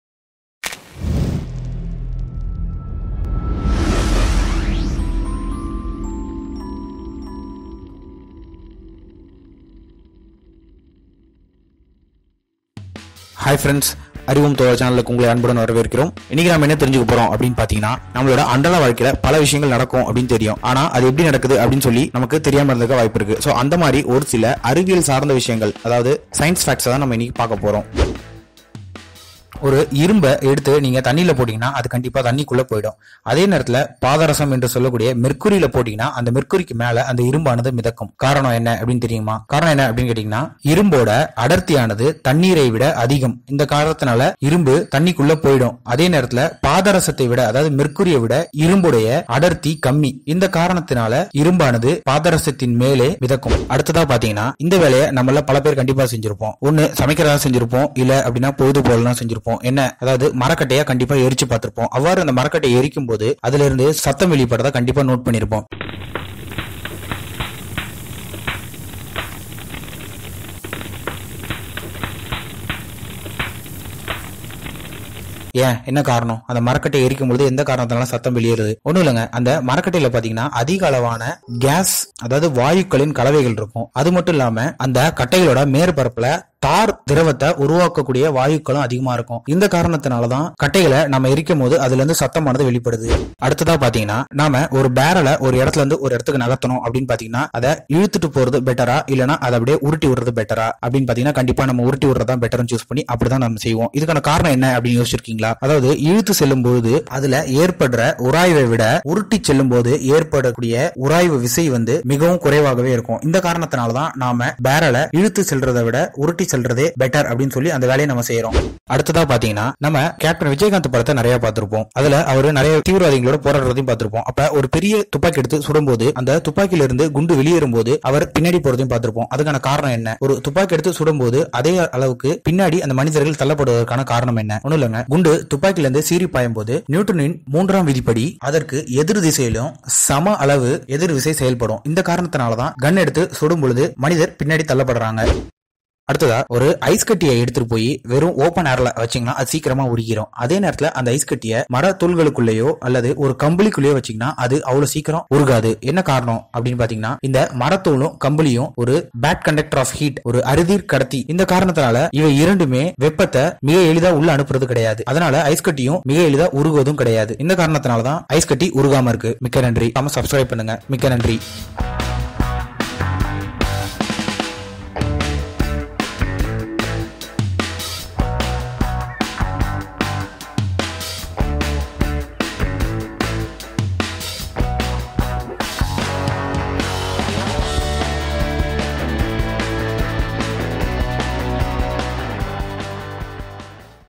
மரக்கட்டை எரியும் போது வெடிக்கும் சத்தம் கேட்கிறதே ஏன் ? | அறிவியல் உண்மைகள் -2 | ARIVOM THOZHA
This video contains quick scientific facts that might make us wonder how day- to-day things happen around us, and will make us look at things in a different way.